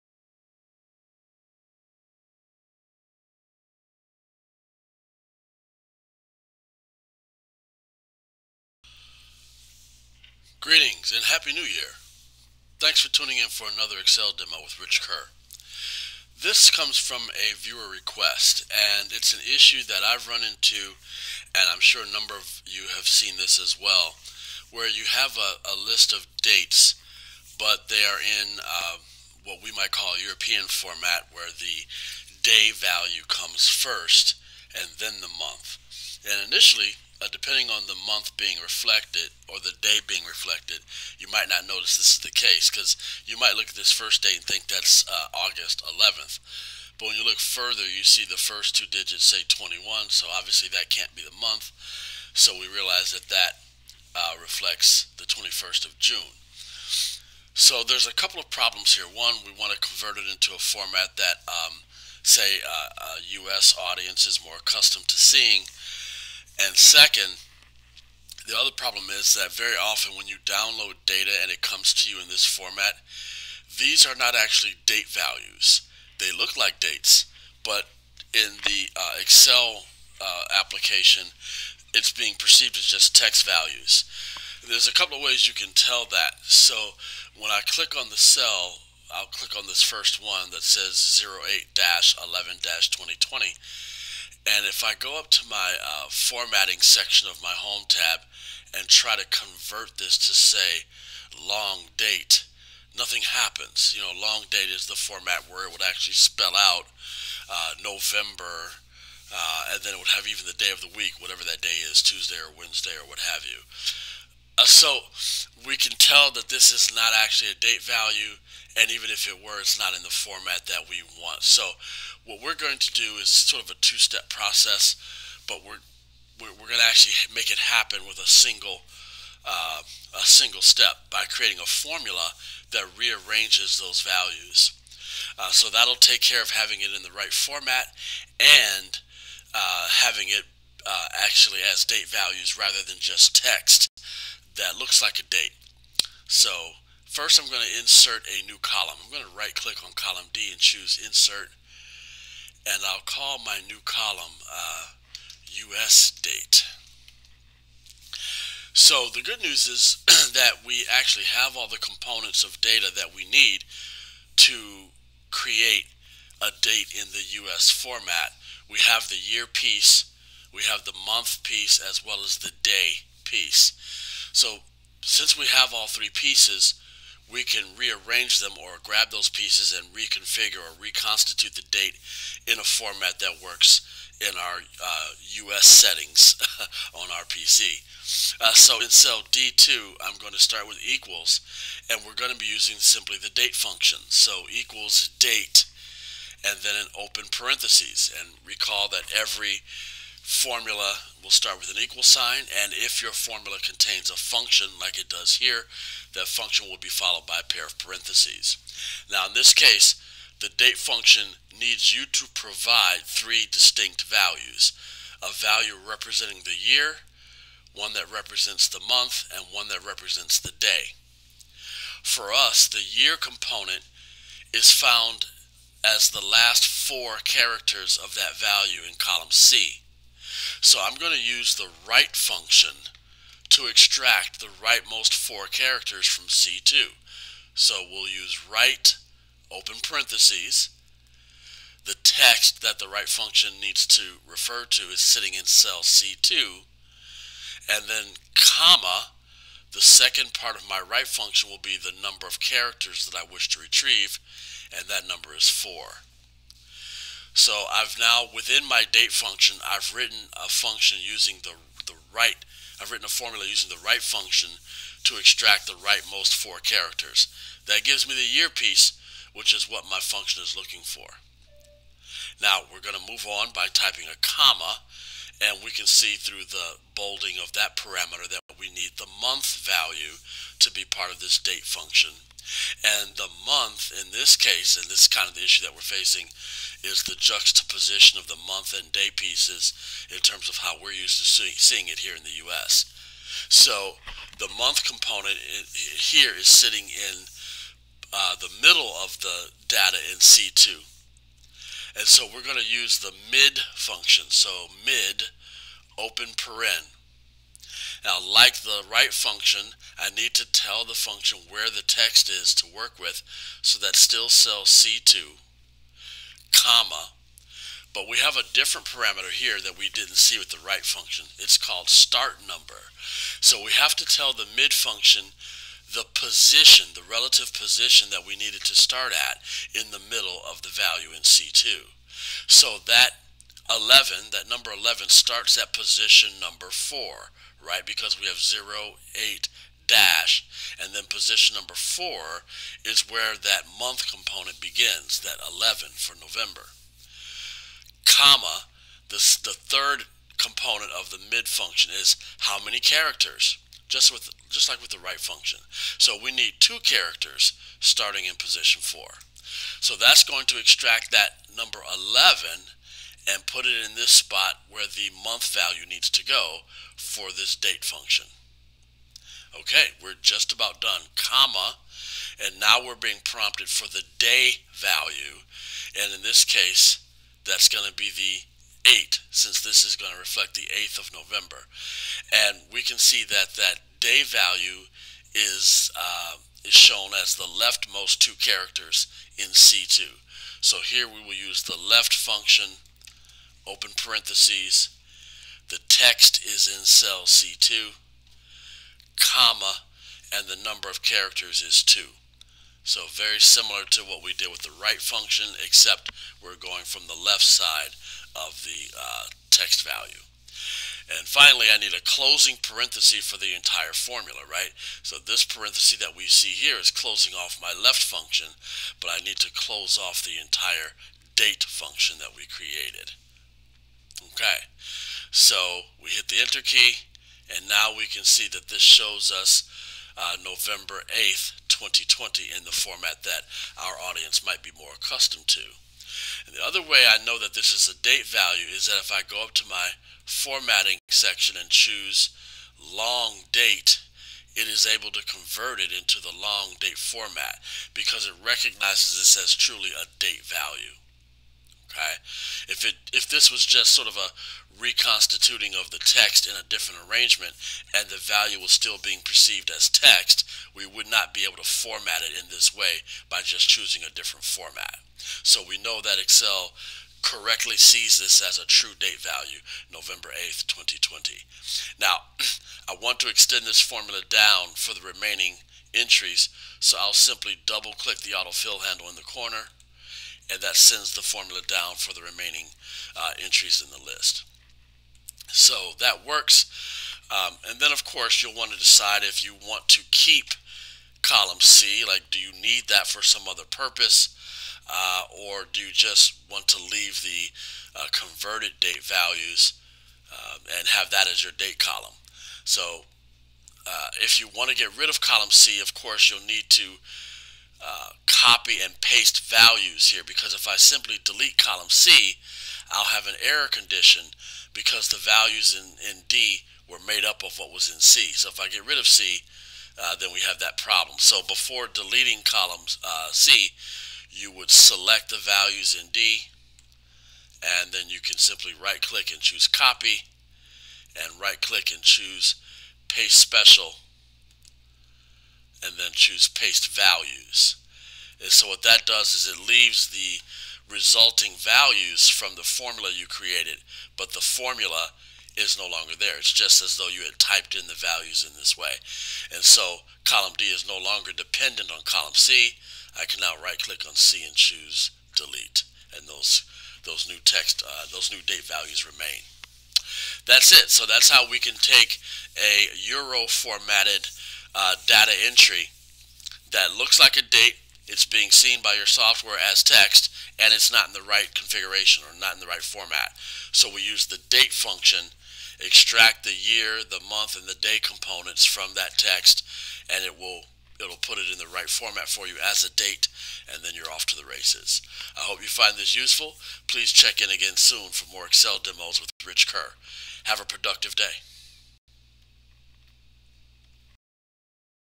Adjust Euro Dates in Excel
Use the DATE function to re-arrange the month, day and year components of text showing a date in "European" format.  Convert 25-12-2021 to 12/25/2021 for example.